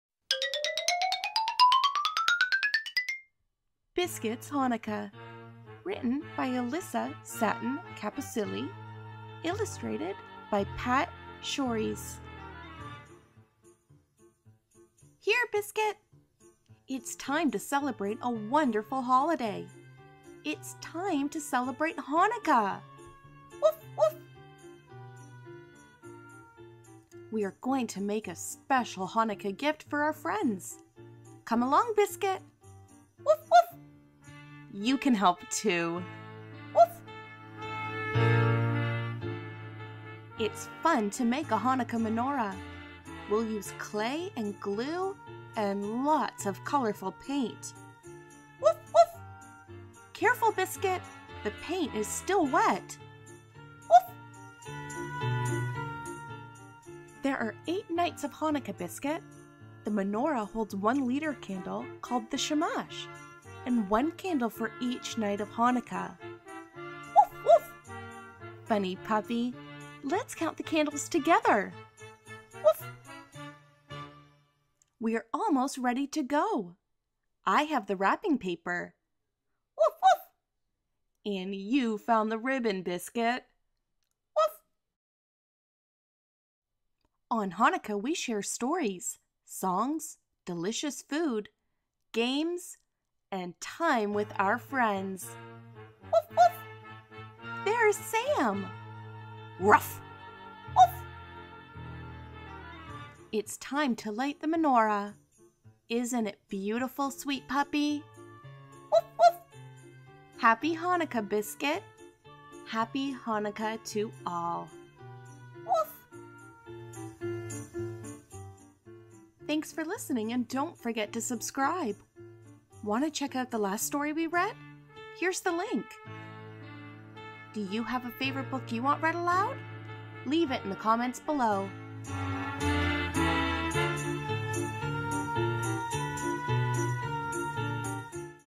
Biscuit's Hanukkah - Kids Books Read Aloud
Biscuit's Hanukkah
Author - Alyssa Satin Capucilli
Illustrator - Pat Schories

Come along with Biscuit as he makes a beautiful menorah to celebrate Hanukkah. It's a great time for stories, songs, food and friends!